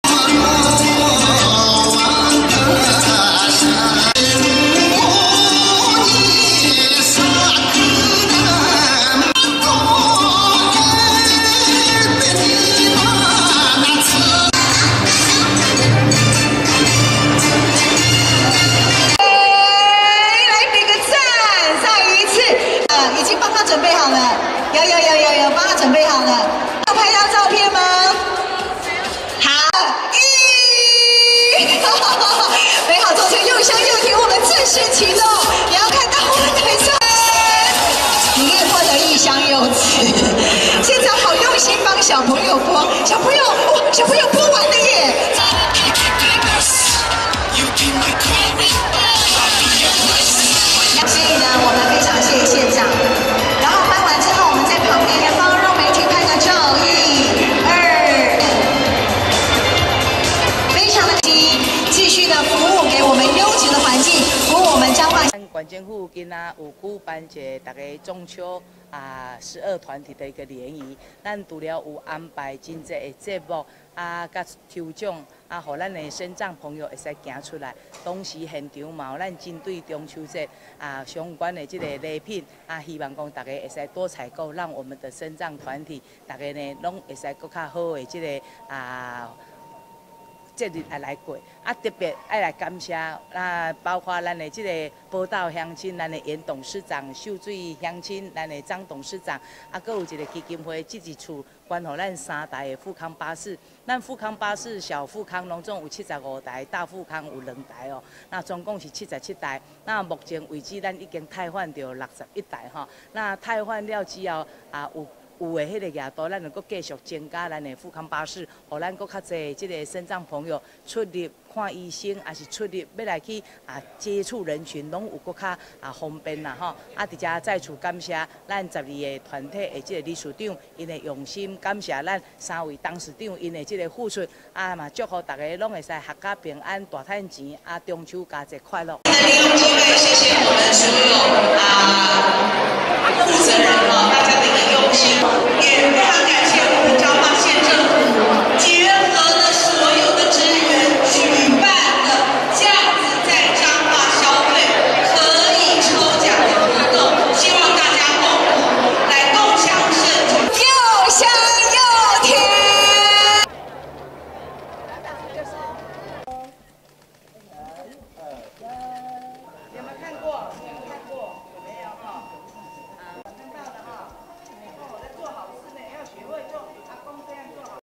彰化縣中秋慶典捐贈3輛復康巴士 助力無障礙環境
（有影片）／彰化縣中秋慶典捐贈3輛復康巴士 助力無障礙環境
（觀傳媒彰化新聞）【記者陳雅芳／彰化報導】中秋佳節的腳步臨近，彰化縣政府於今(7)日下午在彰化縣體育館舉辦「美好中秋．柚香柚甜」，113年度彰化縣身心障礙者中秋節活動，邀請全縣身心障礙團體共同參與盛會，吸引約1,200名民眾到場，彰化縣長王惠美說，希望透過此活動增加身心障礙朋友及家庭的社會參與機會，感受中秋團圓氛圍。